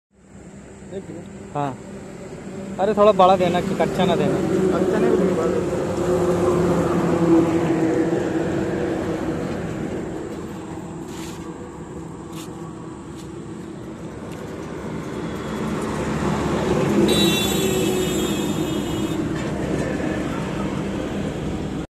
Kalmi Mango Variety in India, कलमी आम की क़िस्म
Kalmi Mango Variety in India, कलमी आम की क़िस्म mango variety in up 'Chausa', 'Dusheri', 'Fazli', 'Gulab Khas', 'Langra', 'Mallika' and 'Amrapali'.